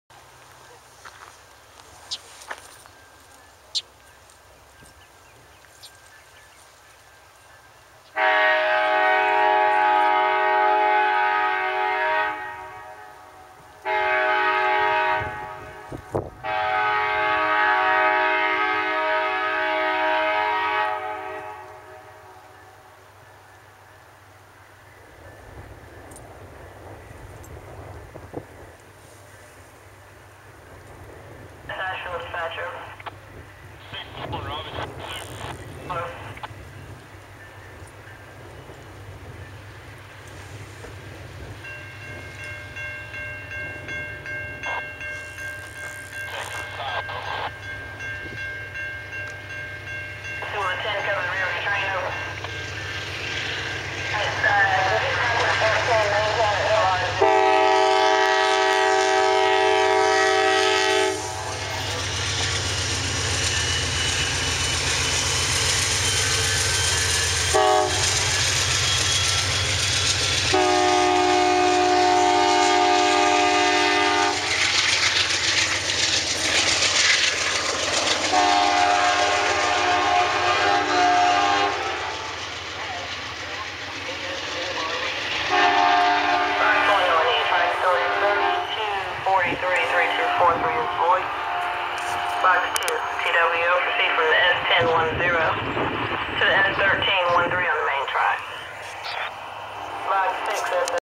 Amtrak 74
Amtrak 74 is departing Salisubry